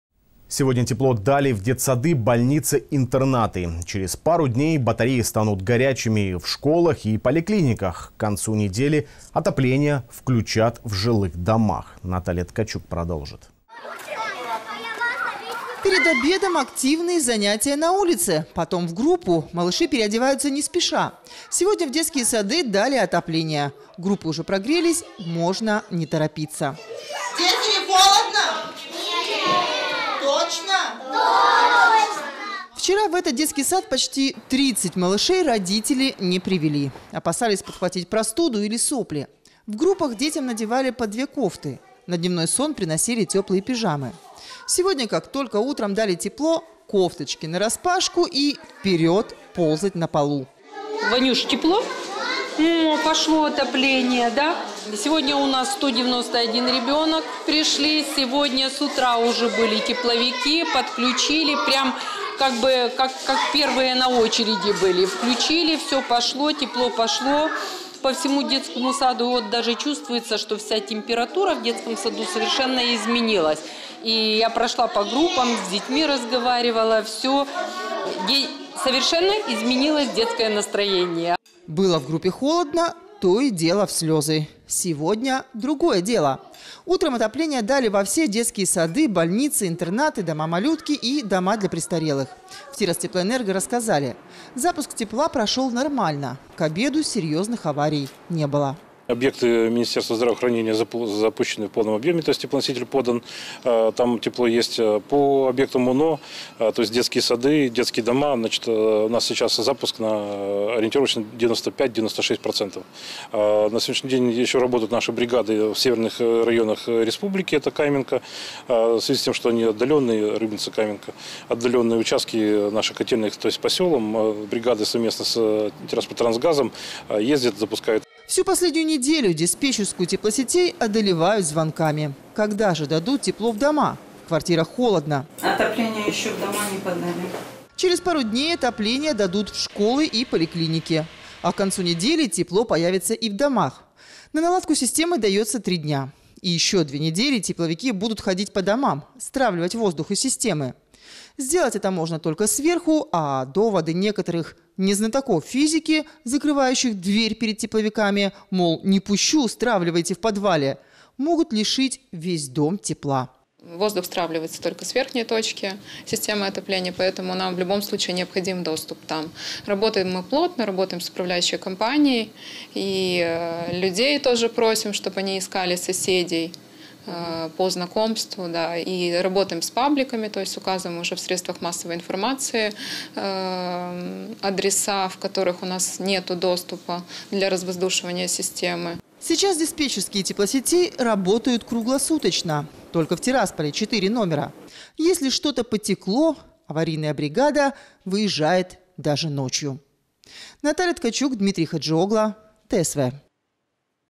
Тепло дали в детсады и больницы
Сегодня тепло дали в детсады, больницы, интернаты. Через пару дней батареи станут горячими в школах и поликлиниках. К концу недели отопление включат в жилых домах.